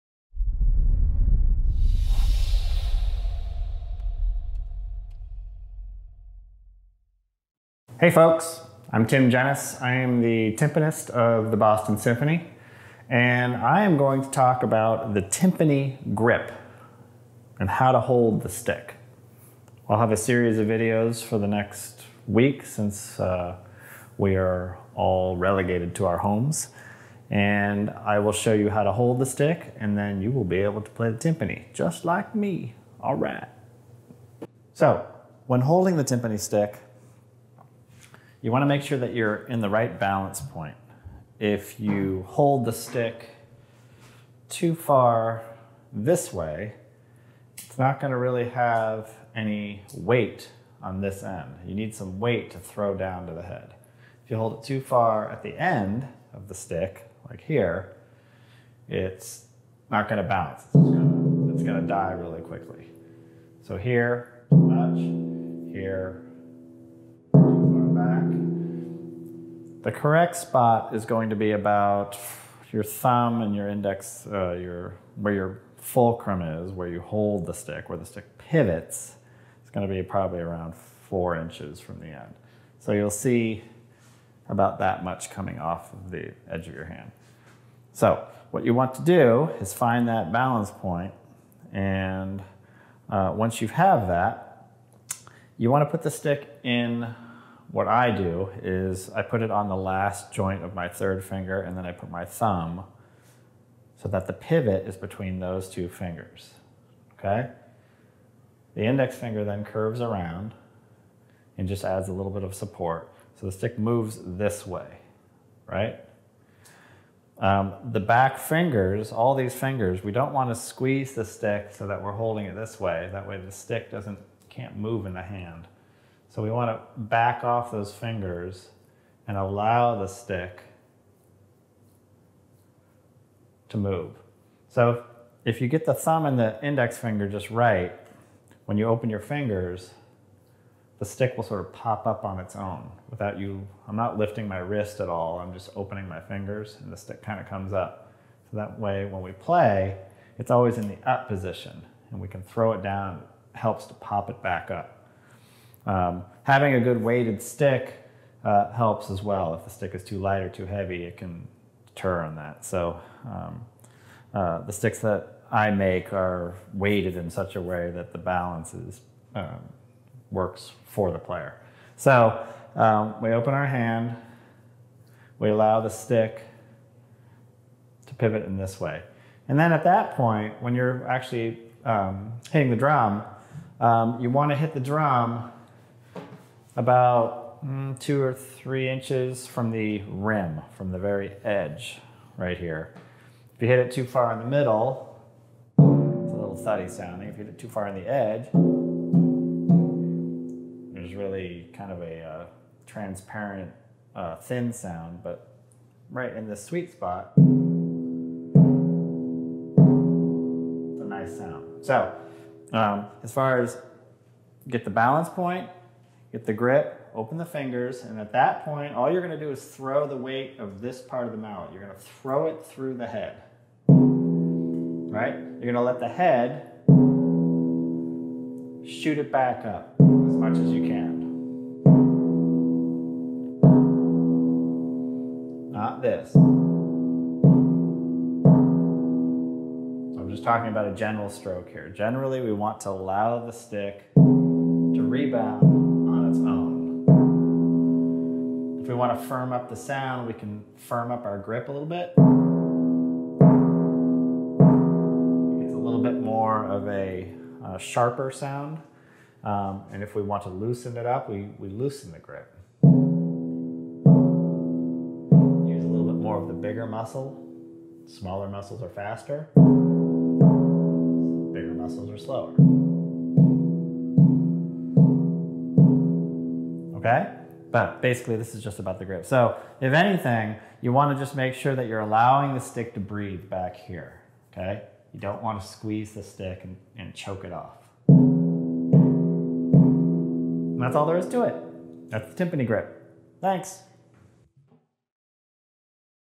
BSO Homeschool | Tim Genis
BSO timpanist Tim Genis demonsrates the timpani stroke.

Find many more online musical resources like this one

Follow us on all of our social channels!